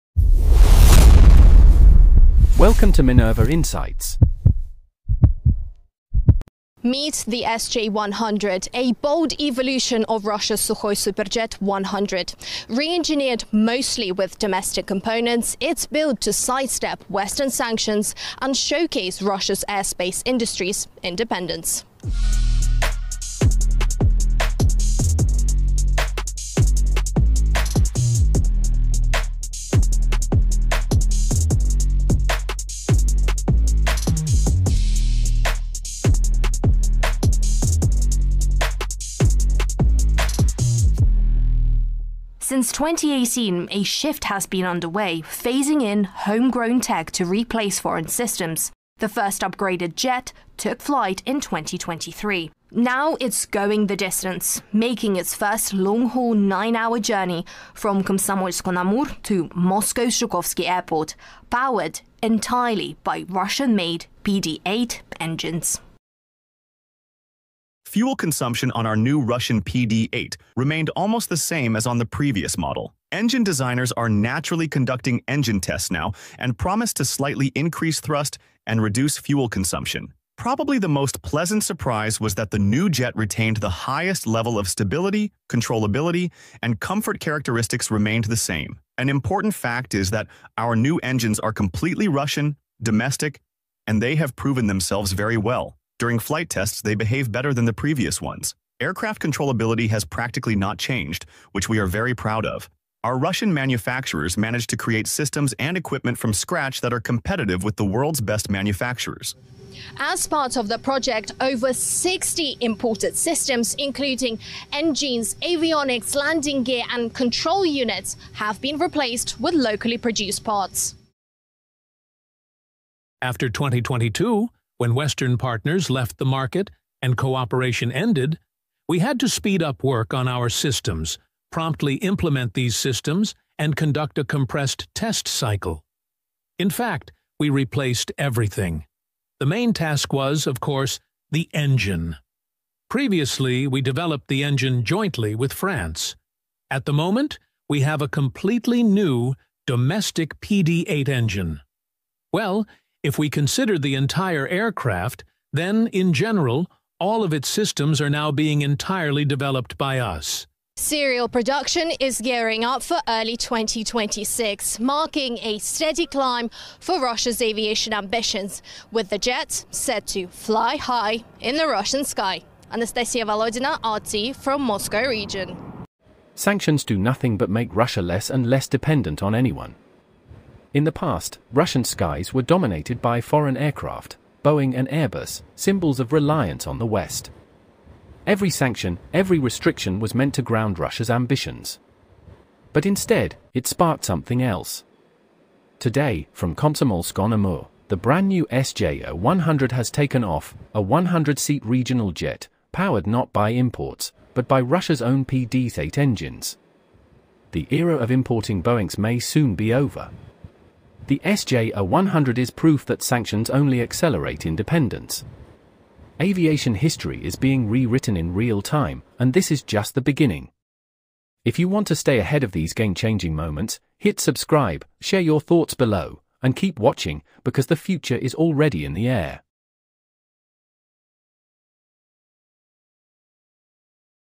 Russia’s New SJ-100 : Full Tour of Boeing’s Replacement
Sanctions do nothing but make Russia less dependent on anyone.

The brand-new SJ-100 aircraft, powered by Russian PD-8 engines, has successfully taken off from Komsomolsk-on-Amur. With a capacity of 100 passengers, this jet is designed to replace Boeing and Airbus in Russian skies.

From the cockpit to the engines, this is your exclusive look at the future of Russian aviation.